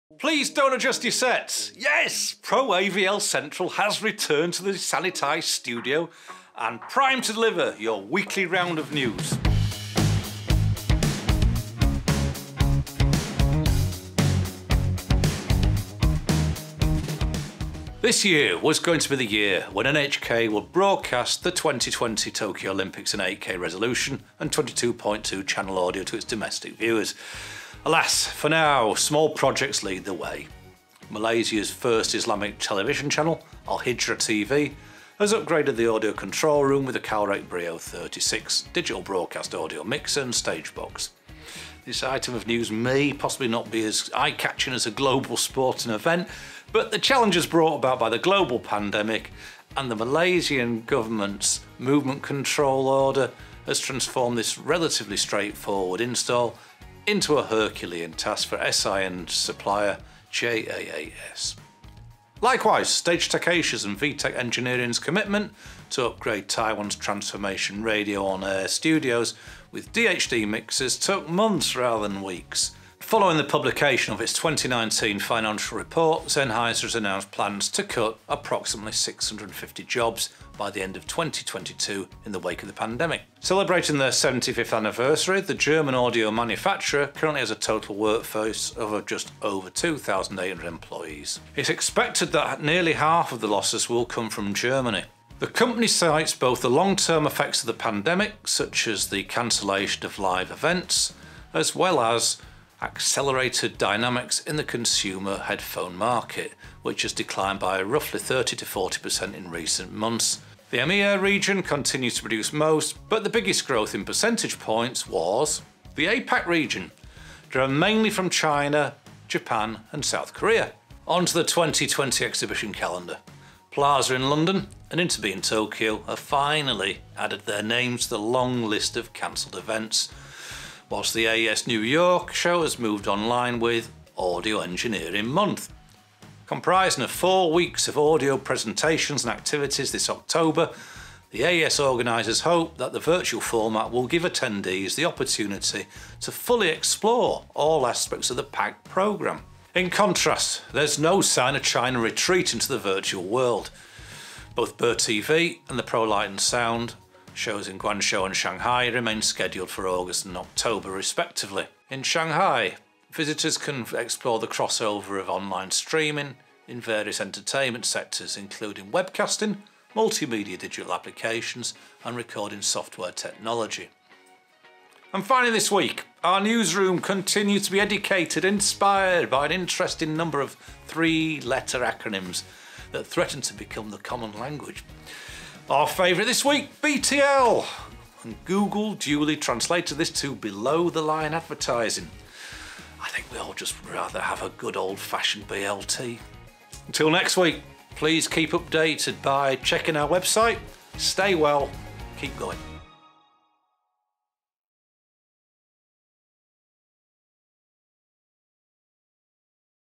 Pro AVL Central This Week | Complying to the new normal | 17/07/2020
Complying to the new normal, we have added an overhead boom mic, applied disinfectant and sanitised our studio in order for us to continue with our weekly news round up.